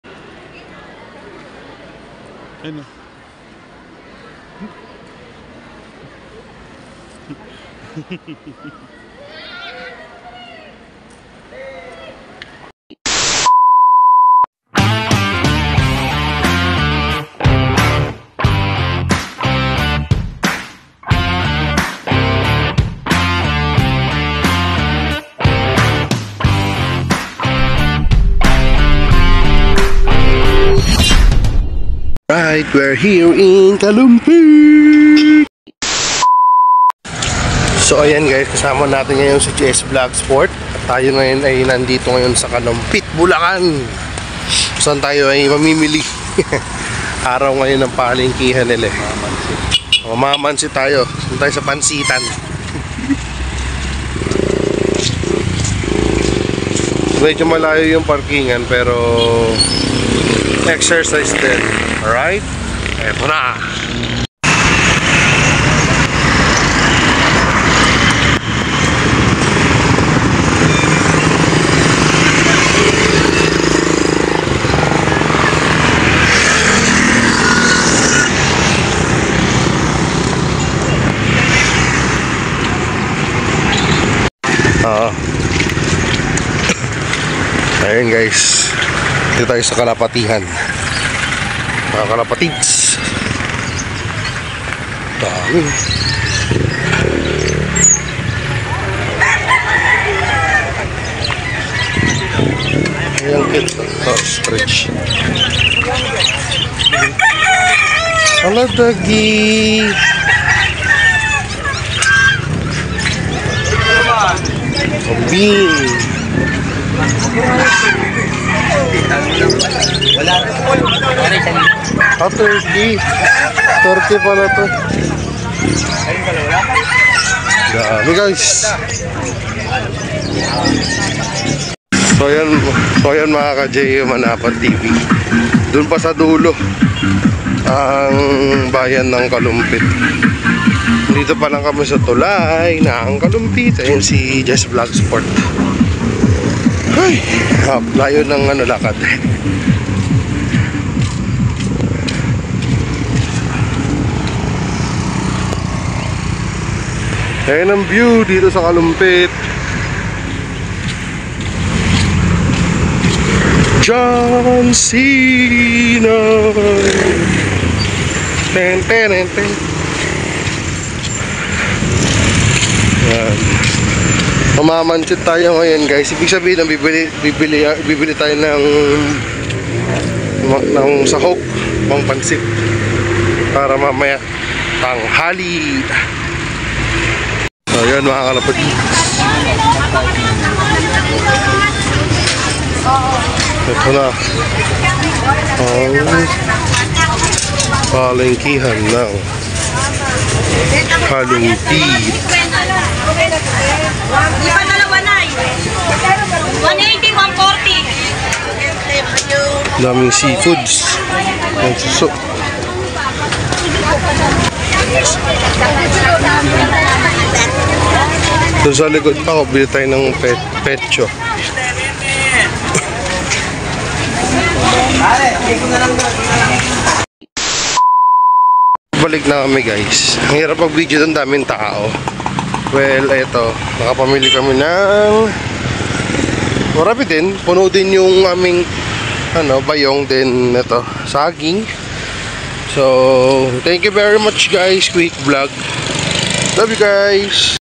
Very Short Vlog: Pumunta sa Calumpit
Jessvlogport@ibapa and Jeomanabattv goes in Calumpit Bulacan